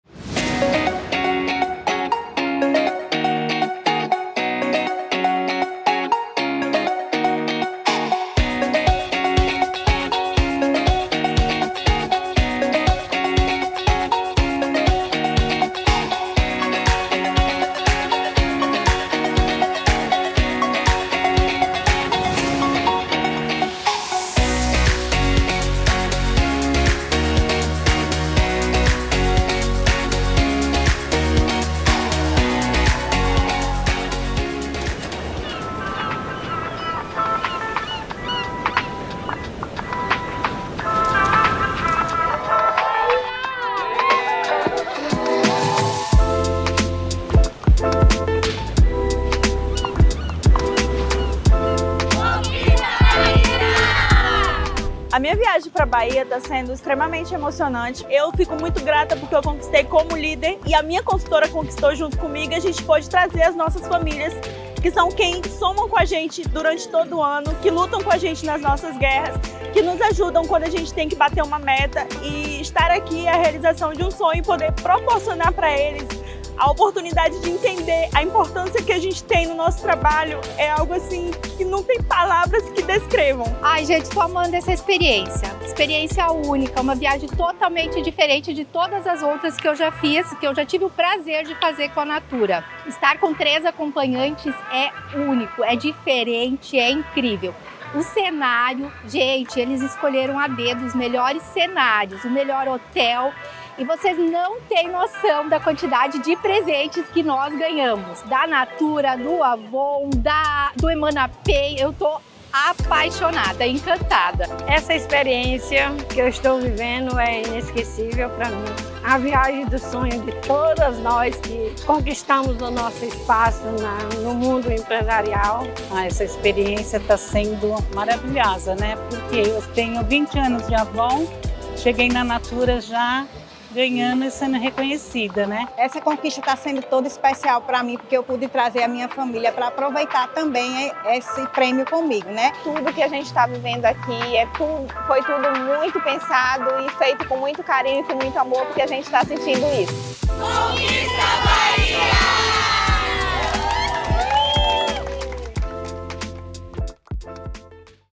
Conquista Bahia
Nossas Consultoras de Beleza e Líderes de Negócios vencedoras da campanha de Natal Brilhou, Conquistou foram encantadas pela magia da Bahia, e ainda puderam curtir essa viagem com 3 acompanhantes cada. 😍 Vamos relembrar os melhores momentos? Dê play e descubra como foi essa aventura. 🎉✈️

Vídeo com uma seleção dos melhores momentos de uma viagem à Bahia, na qual Consultoras de Beleza e Líderes de Negócios desfrutam de experiências inesquecíveis ao lado de familiares e amigos.